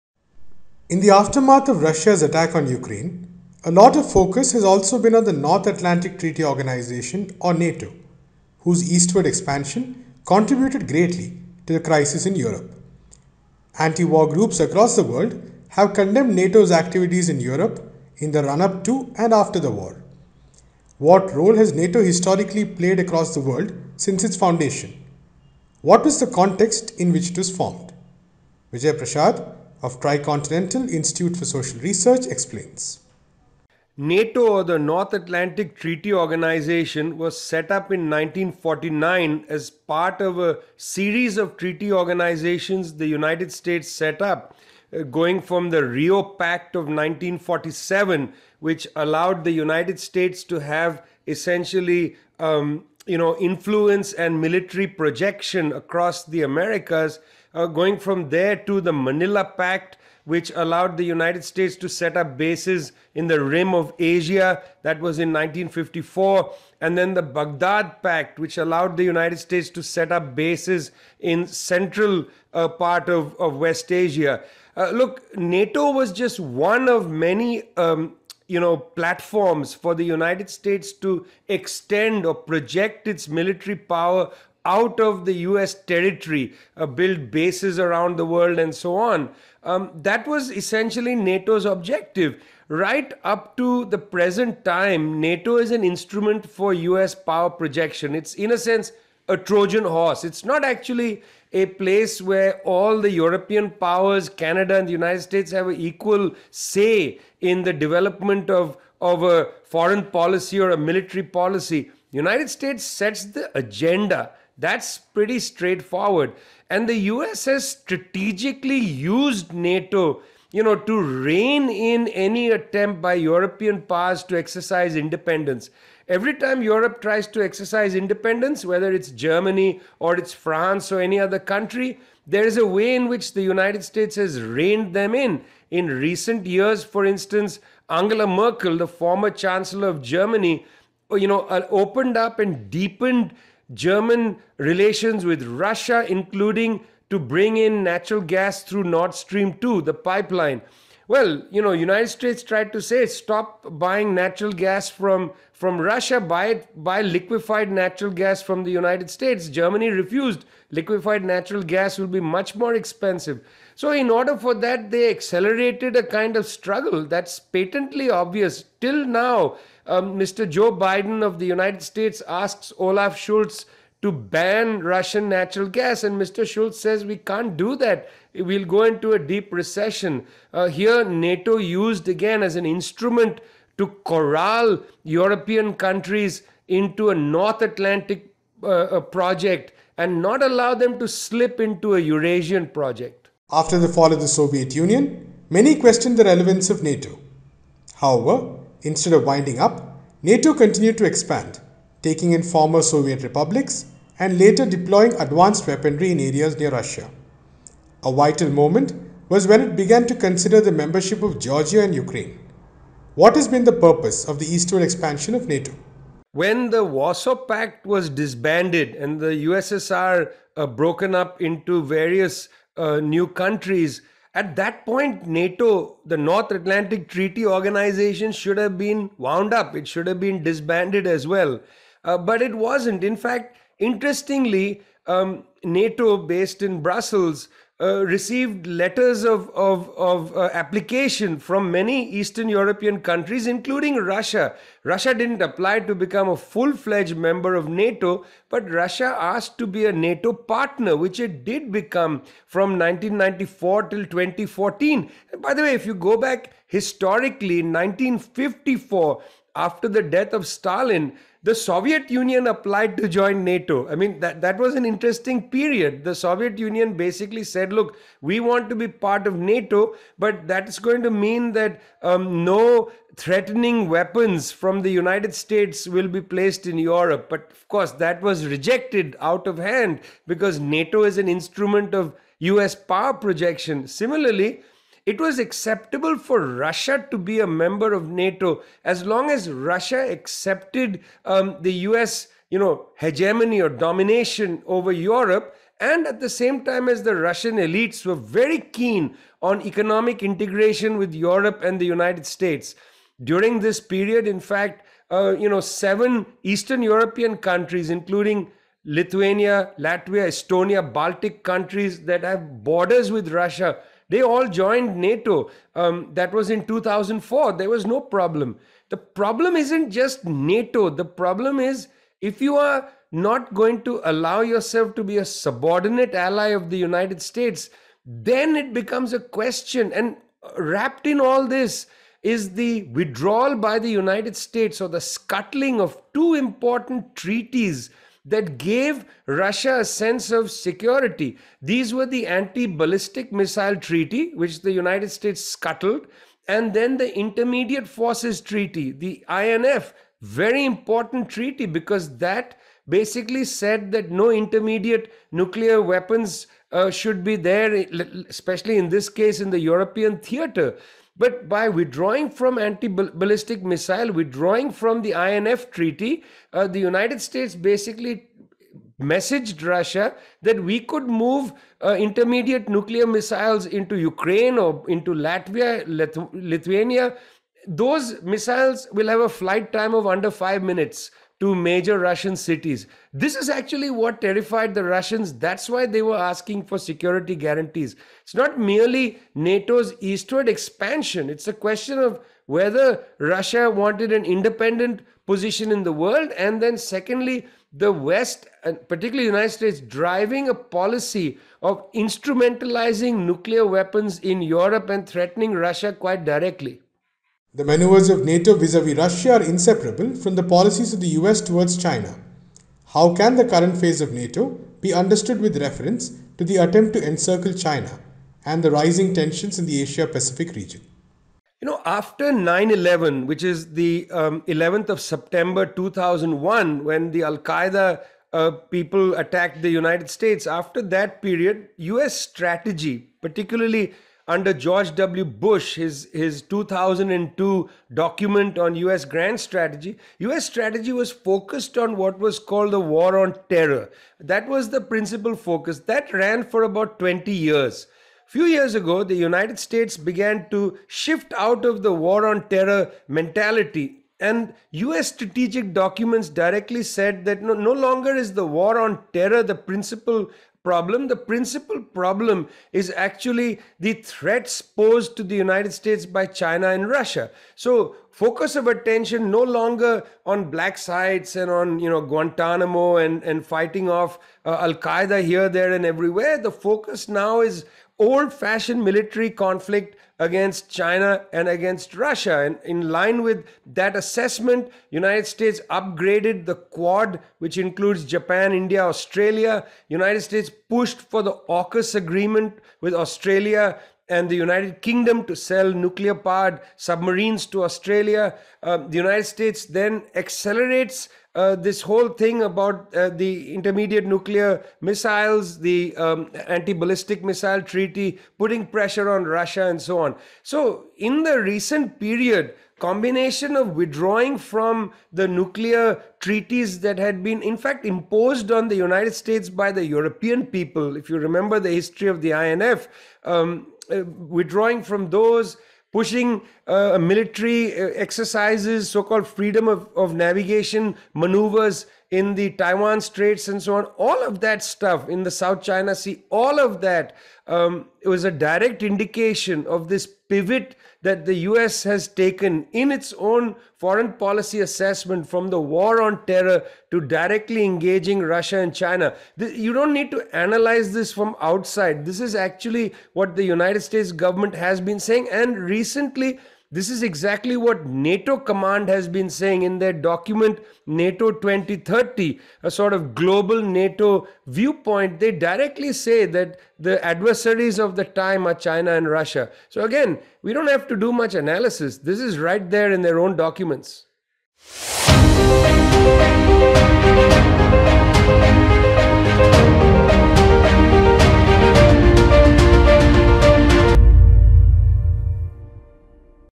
Why is NATO even a thing?
Vijay Prashad of Tricontinental: Institute for Social Research talks about the historic role played by NATO as a tool of US strategy. He explains the context in which it was founded, its expansion in Europe that escalated tensions, and its larger orientation against Russia and China.